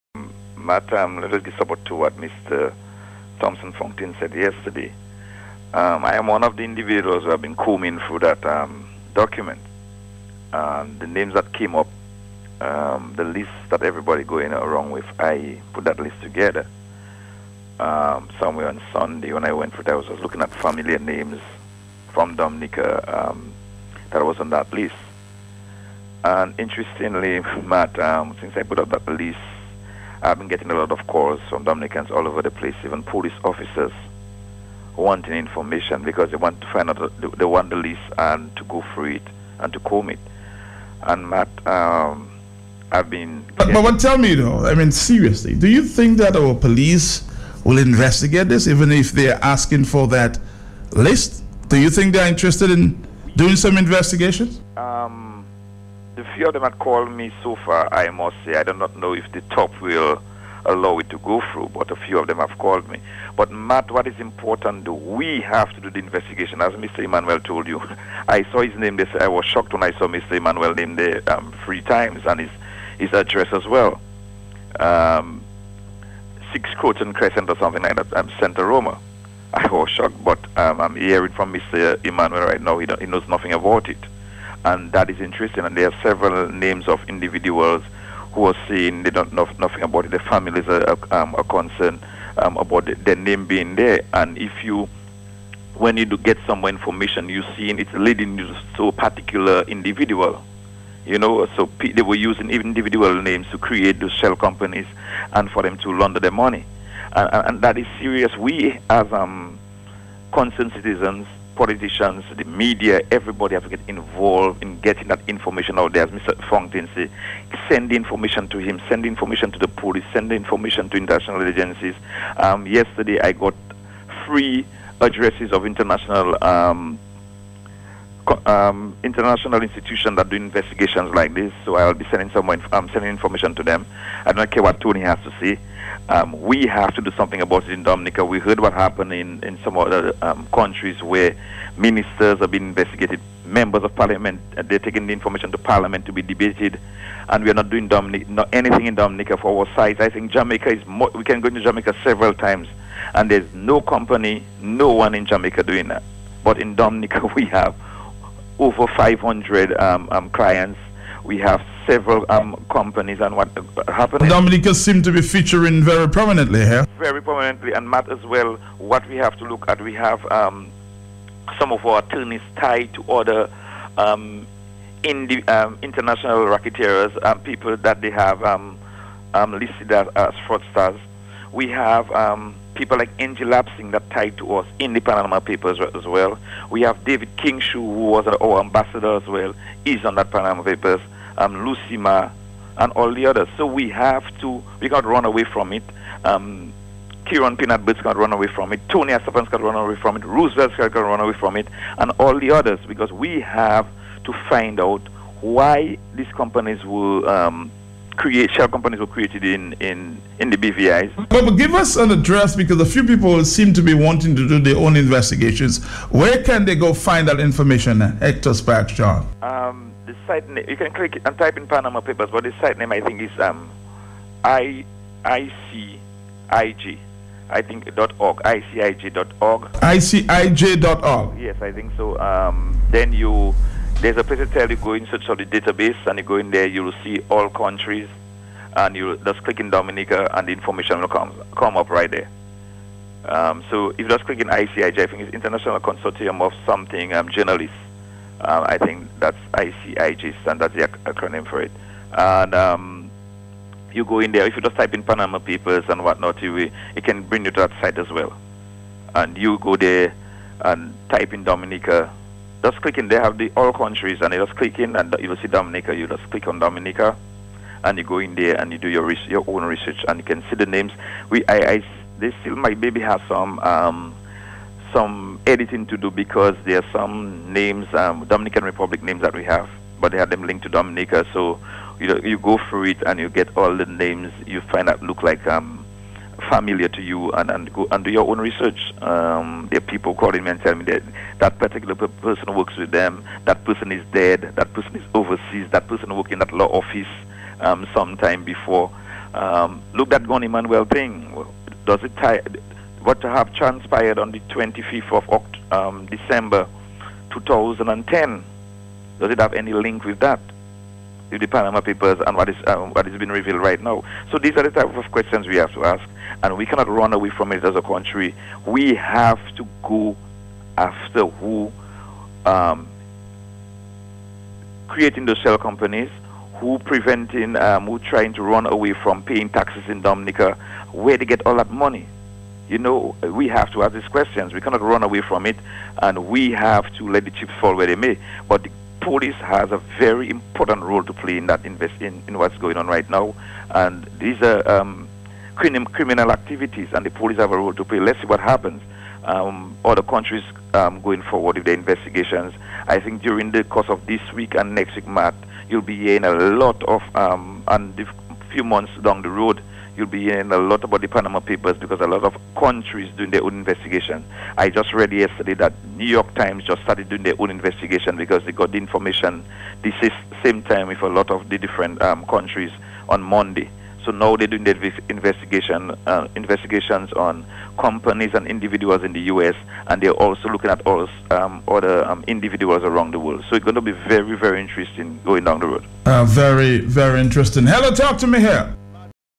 Parliamentary Representative of Salisbury Constituency views on the Panama Papers
Parliamentary Representative of Salisbury Constituency Hector John views on the Panama Papers. The Panama Papers is the largest ever release of secret offshore companies and the people behind them. The 11-million documents leaked from a Panamanian law firm Mossack Fonseca, show how some wealthy people use offshore firms to evade tax and avoid sanctions.